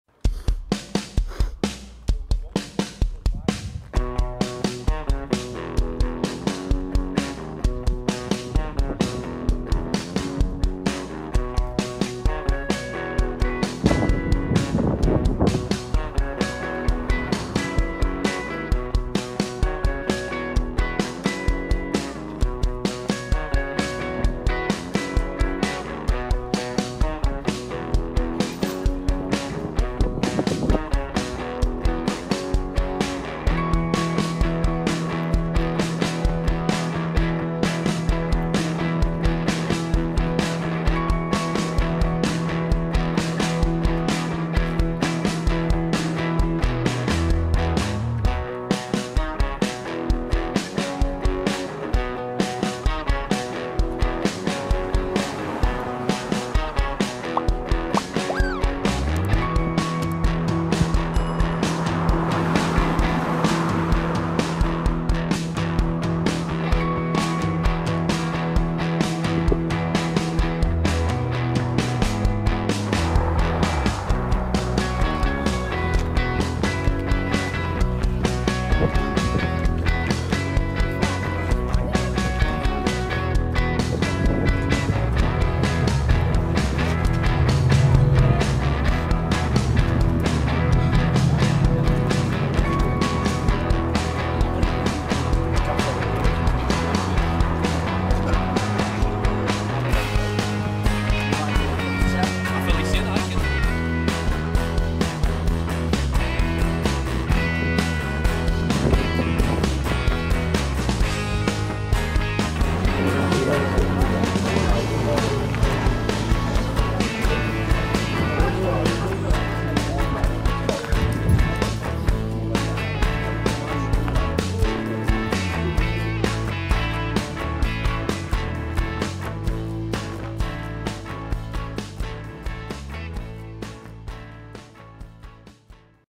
Cars and Coffee
Another varied selection from my Cars and Coffee night at Millie's